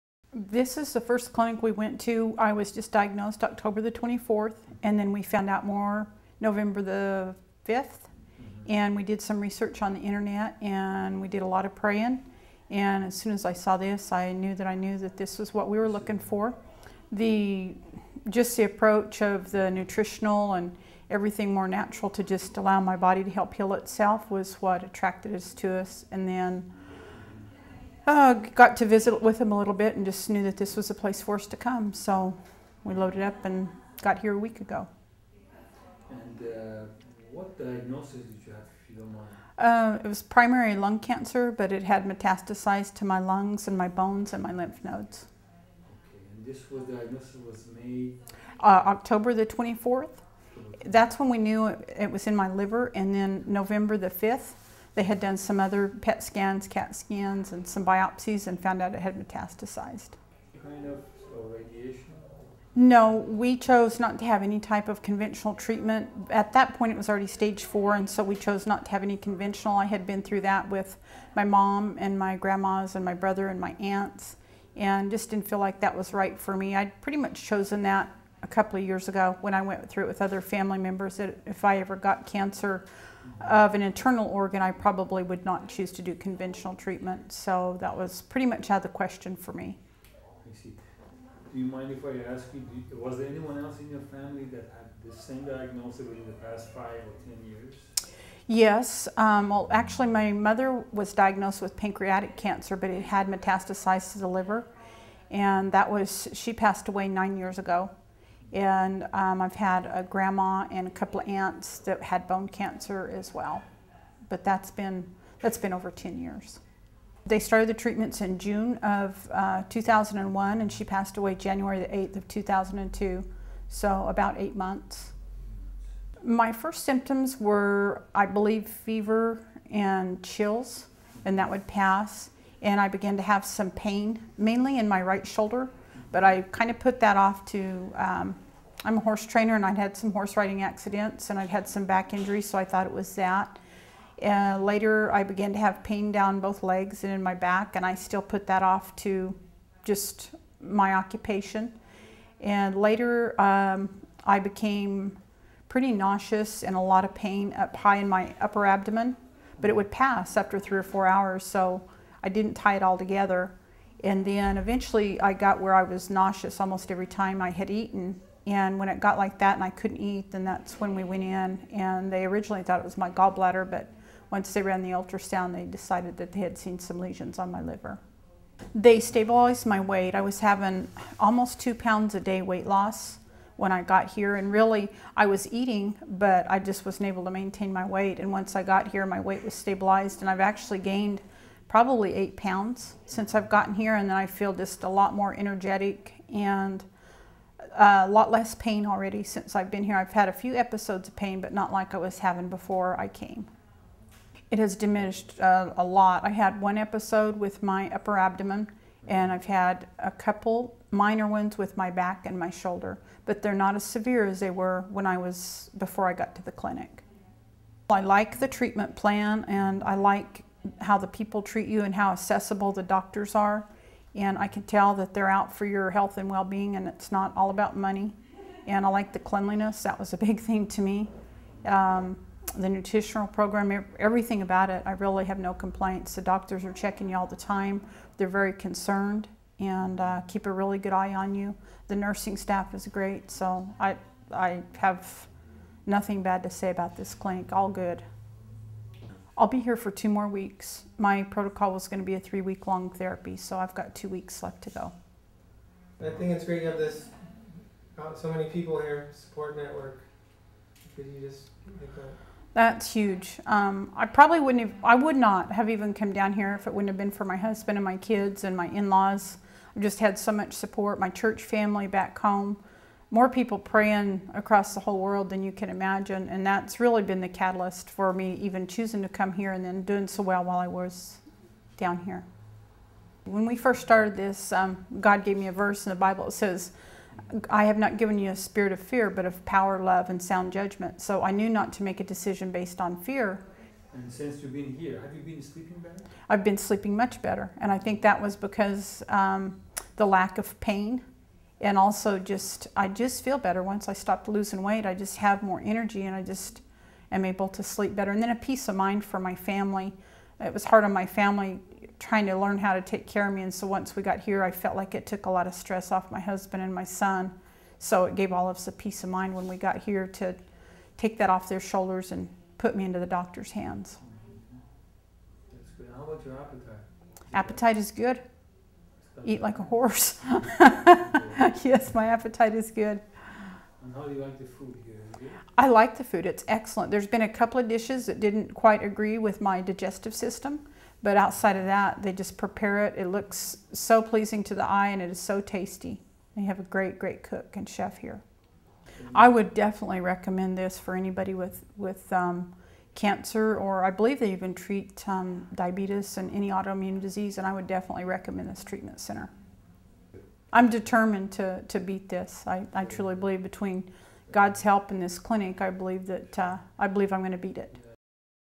Patient interview 1 at Hope 4 Cancer Baja
A stage 4 cancer patient describes her experience at Hope 4 Cancer Baja